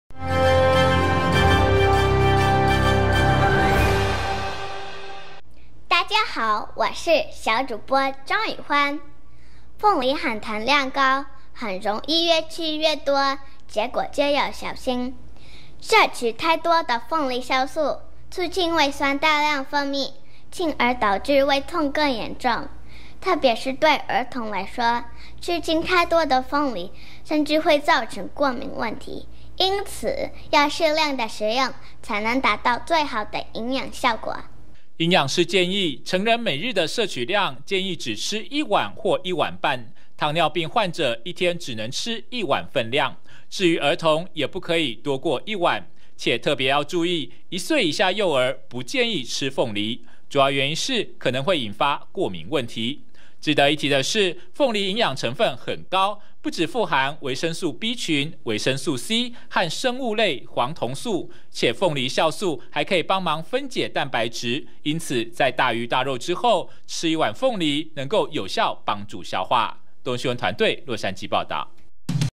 小心鳳梨吃太多！幼兒恐引發過敏問題
鳳梨是營養成分高 且 美味的水果。不過，吃多了會造成什麼問題？就讓 Toyota 榮譽贊助的，2021年「東森幼幼小主播選拔賽」，馬上由 七到九歲的「酷比組小主播」，為全美觀眾報導，最新 科普知識。

鳳梨 含糖量高，很容易 越吃越多，結果就要小心，攝取太多的 「鳳梨酵素」，促進胃酸 大量分泌，進而導致 胃痛 更嚴重。特別是對 兒童來說，吃進太多的鳳梨甚至會造成 過敏問題。因此，要適量適量的食用，才能達到最好的營養效果。

營養師建議，成人每日的攝取量，建議只吃 一碗或一碗半，糖尿病患者，一天只能吃一碗份量，至於兒童，也不可以多過一碗，且要特別注意，一歲以下幼兒，不建議吃鳳梨，主要原因是，可能會引發 過敏問題。值得一提的是，鳳梨營養成分很高，不只富含 維生素B群、維生素C、和 生物類黃酮素，且鳳梨酵素 還可幫助分解 蛋白質，因此，在大魚大肉之後，吃一碗 鳳梨、能夠有效幫助消化。